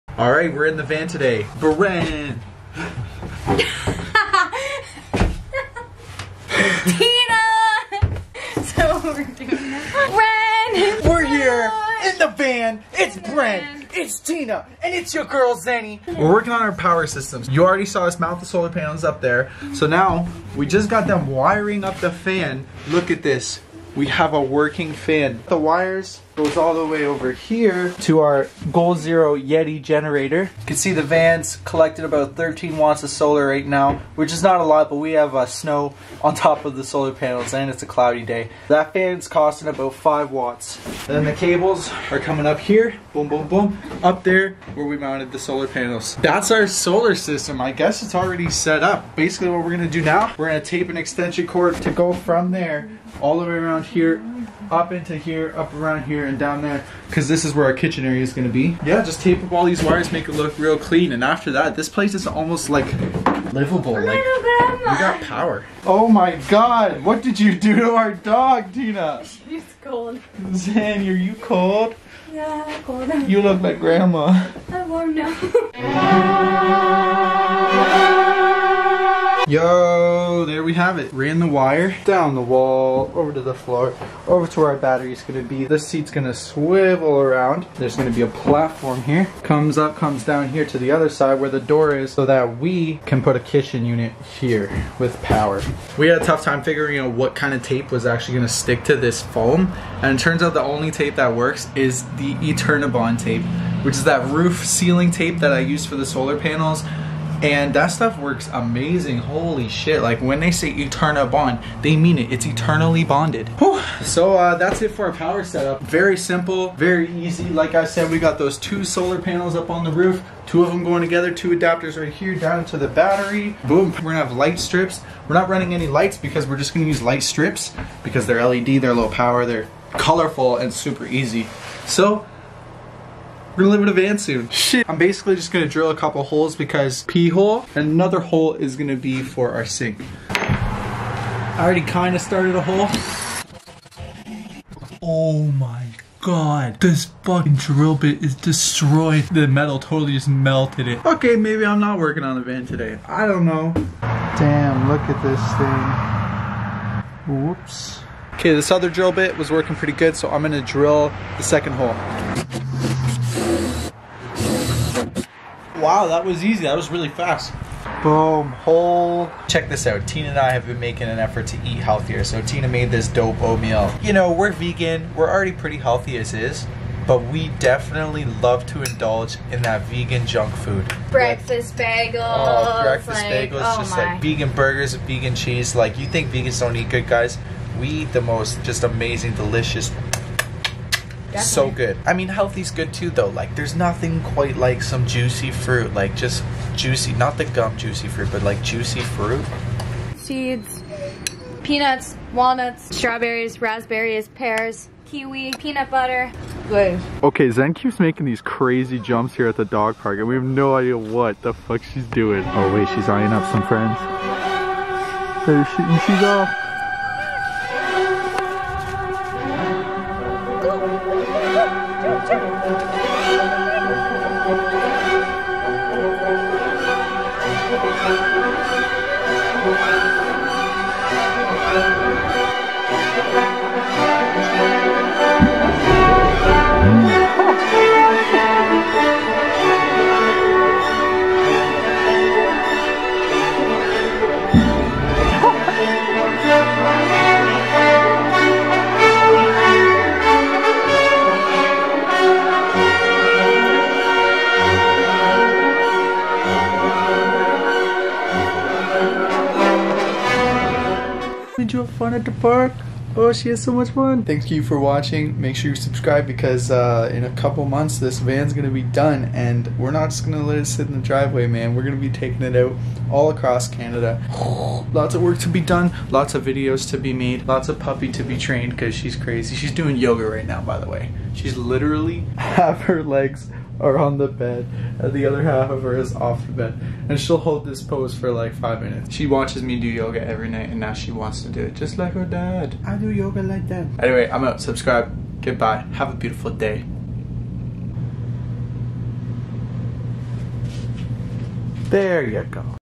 OFF GRID SOLAR POWER VAN!
VLOG #372 - Our Goal Zero Yeti 1400 off grid solar power van generator is all set up, including all the wiring. Big win!